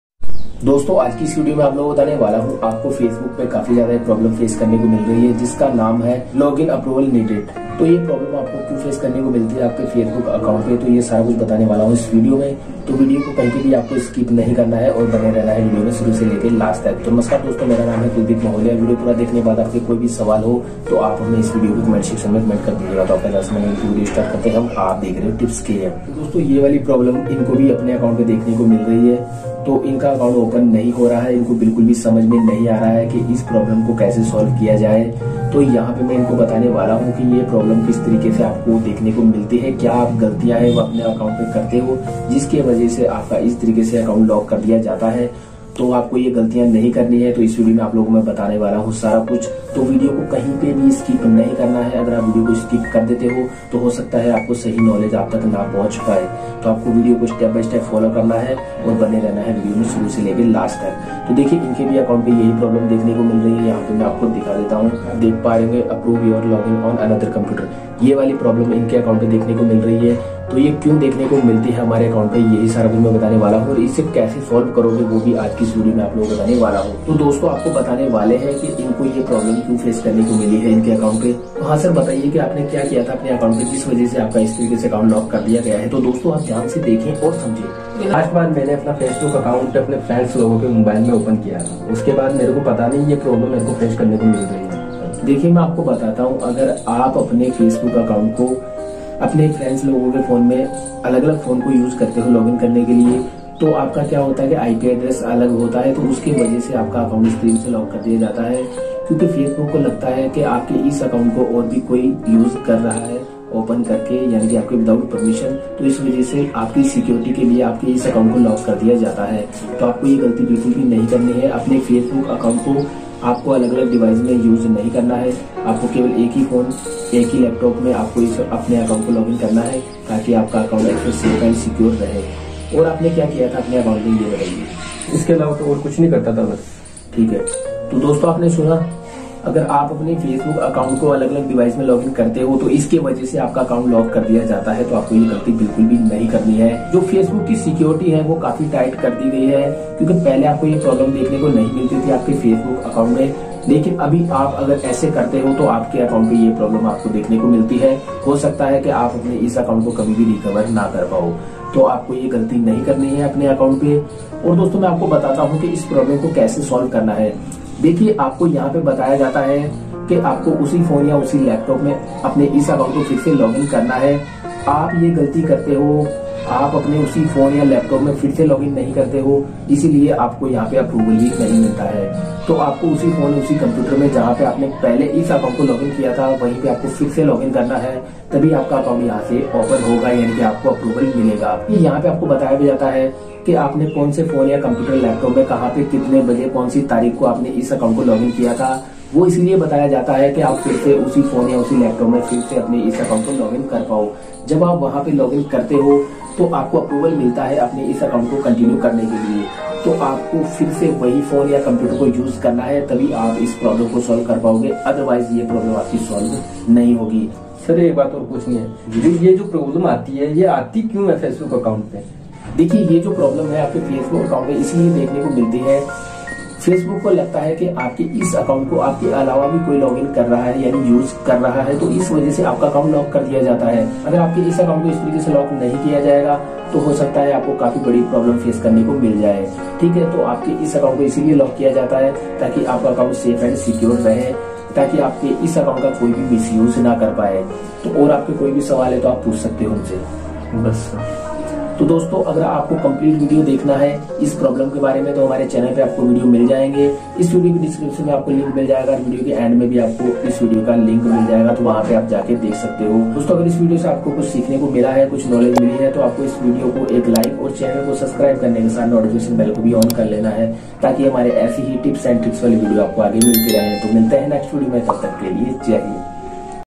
Approve Login Facebook Problem 2021 | Approve Your Login On Another Computer Facebook | TipsKm
Approve Login Facebook Problem 2021 | Approve Your Login On Another Computer Facebook

AAJ KI ISS VIDEO MEIN MAINE BATAYA HAI Ki.
Approve login problem ko aasani se Karen theek
VIDEO KO DHYAN SE🧐DEKHEN AUR SAMJHE🤔

ᕼᗴᏞᏞᝪ🙋DOSTON I am. KႮᏞᎠᎬᎬᏢ ᎷᎪᎻOᏞiᎽᎪ,

VIDEO KE BARE MEIN KUCHH BHI PUCHNA HO TO AAP HAMEN-COMMENT-MEIN POOCHH SAKTE HO?